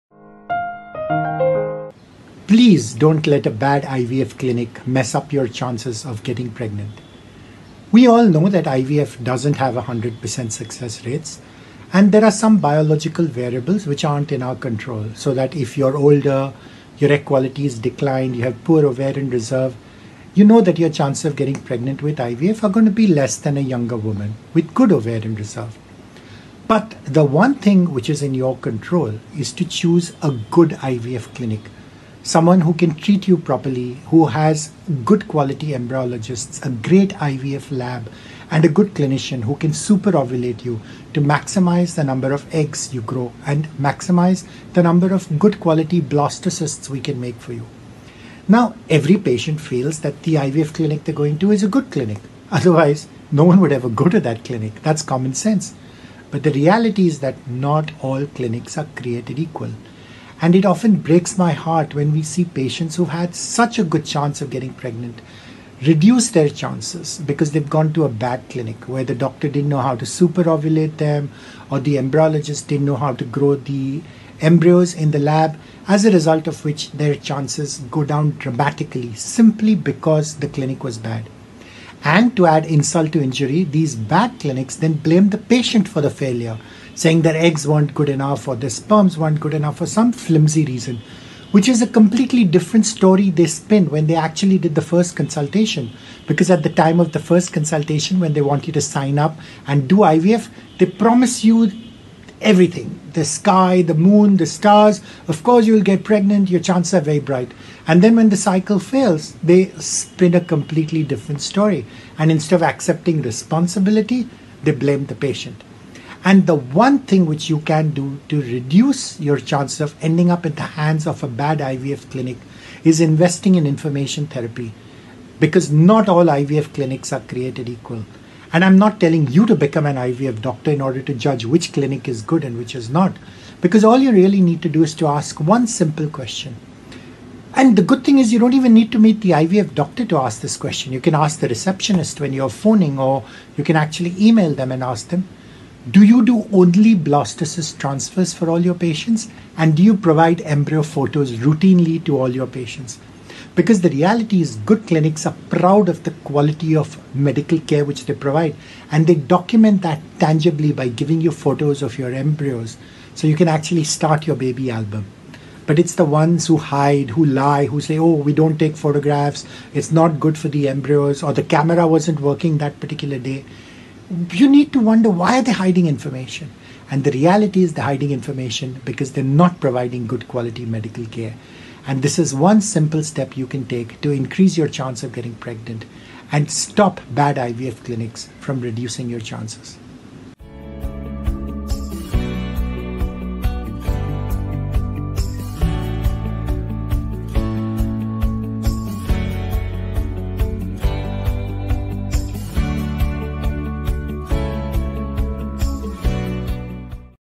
What can mess up your chances of getting pregnant ? IVF Treatment | Pregnancy Tips
Trying to get pregnant? Watch this video to find out what are some of the factors that can make it more difficult for a couple to conceive a baby?